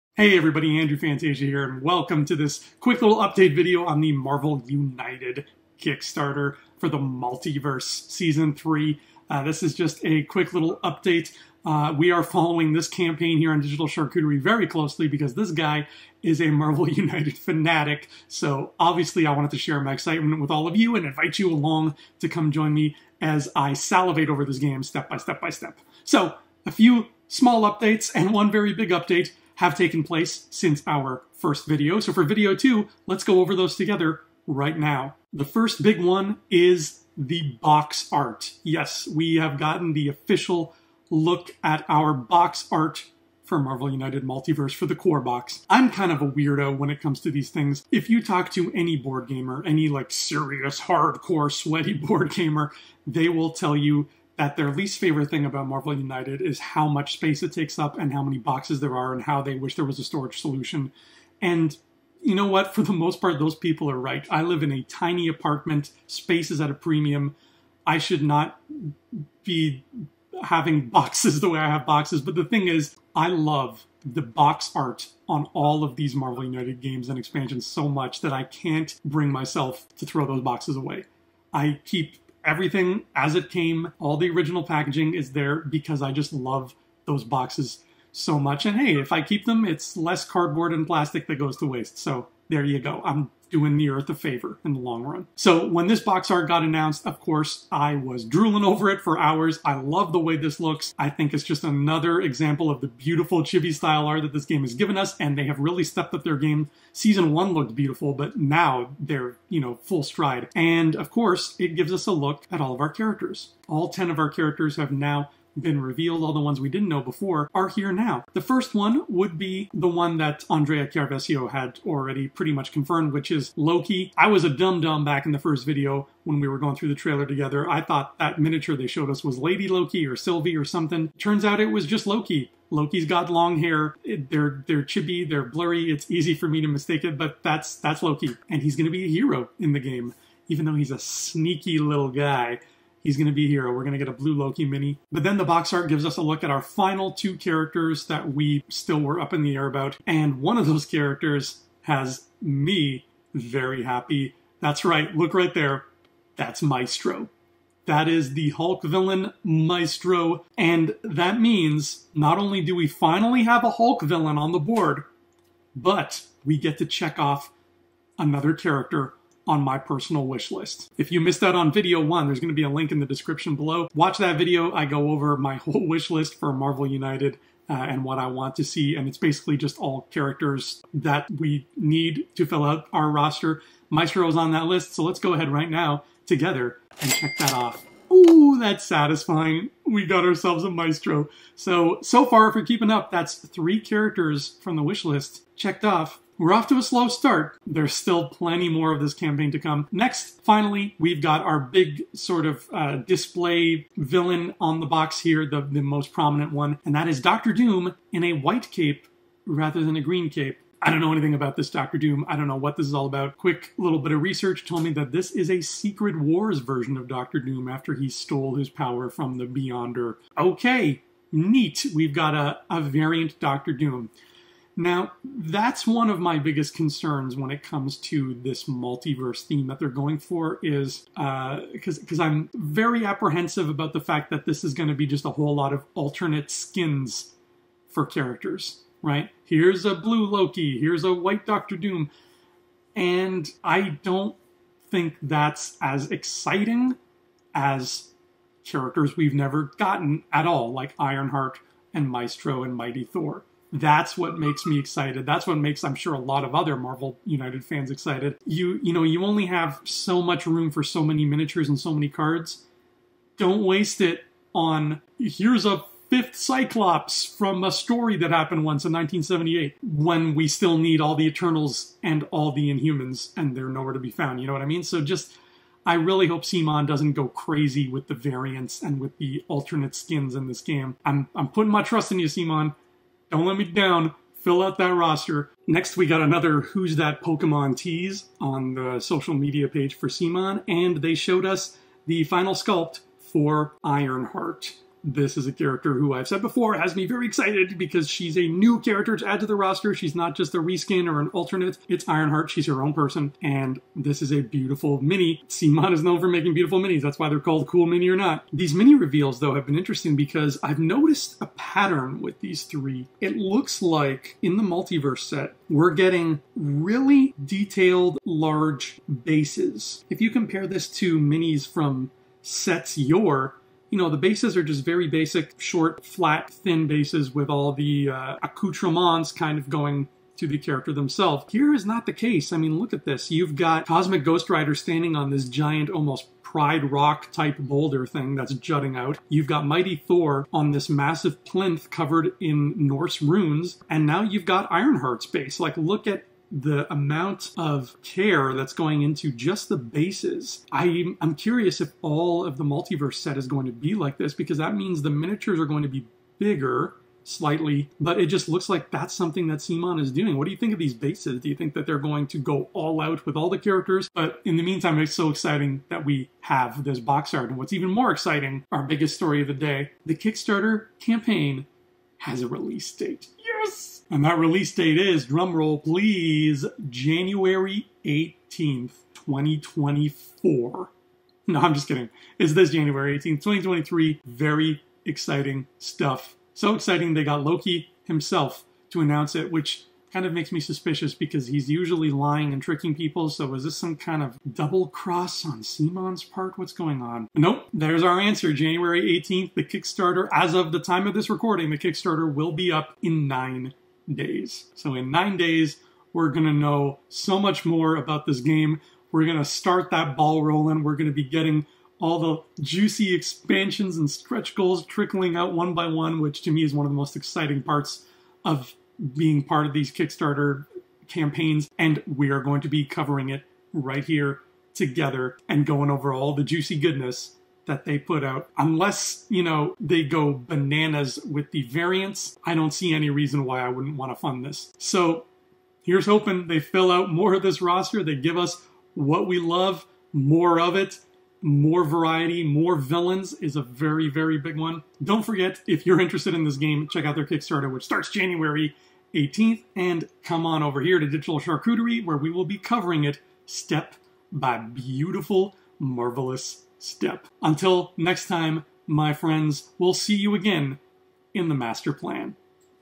MARVEL UNITED: MULTIVERSE | Kickstarter Release Date!
We finally have a release date for the MU: Multiverse Kickstarter...AND IT'S SOOOON!

You can watch our first video (complete with the Wish List to end all wish lists) right